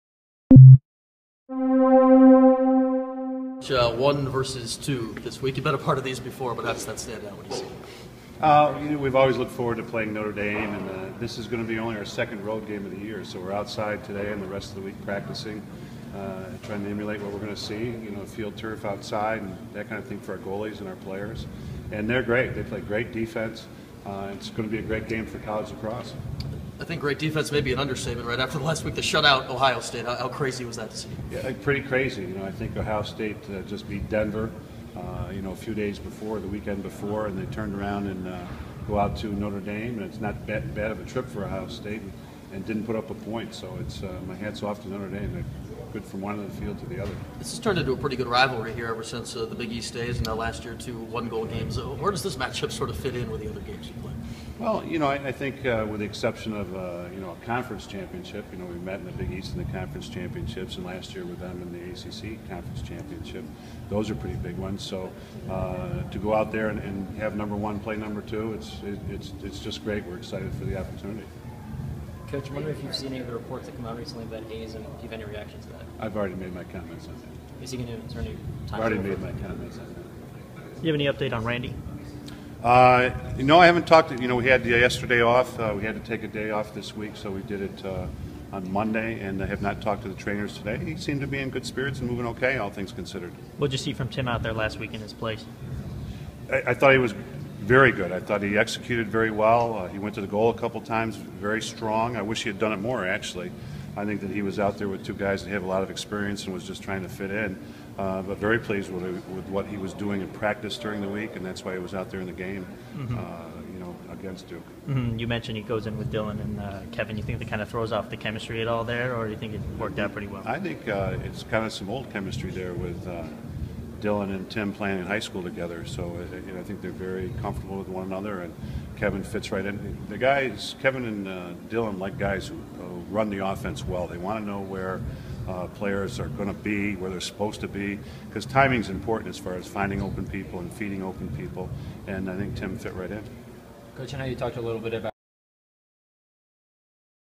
Syracuse lacrosse coach John Desko previews matchup vs. No. 2 Notre Dame
Syracuse lacrosse coach John Desko talks Notre Dame&apos;s defense and Tim Barber, who is filling in for an injured Randy Staats.